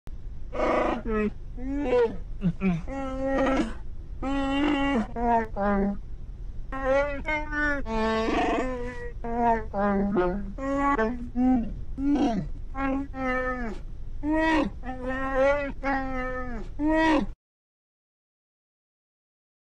Mensaje de Chewbacca
Chewbacca tiene algo importante que decir.
No os lo perdáis.